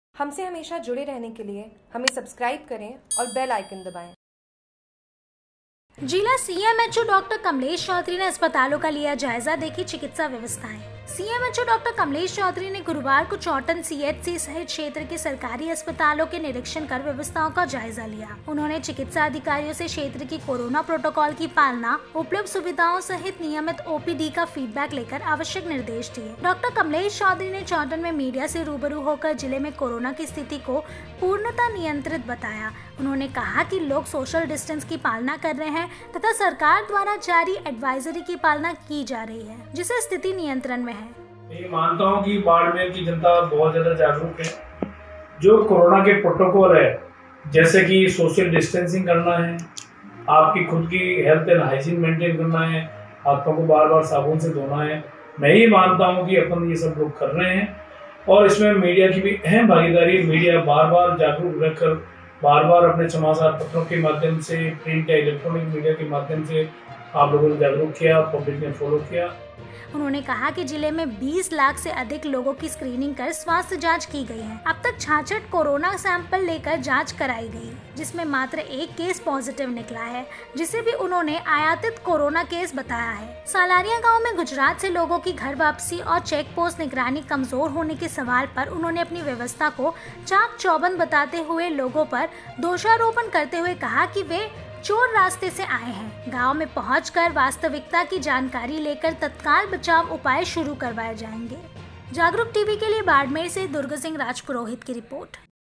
Jagruk TV चौहटन पहुंचे बाड़मेर सीएमएचओ ने किया अस्पतालों के निरीक्षण
Jagruk TV

चौहटन पहुंचे बाड़मेर सीएमएचओ ने किया अस्पतालों के निरीक्षण

बाड़मेर जिले में कोरोना को नियंत्रित बताया

सीएमएचओ ने कहा कि जिले में स्थिति सामान्य

अब तक 66 कोरोना के सेंपल लिए गए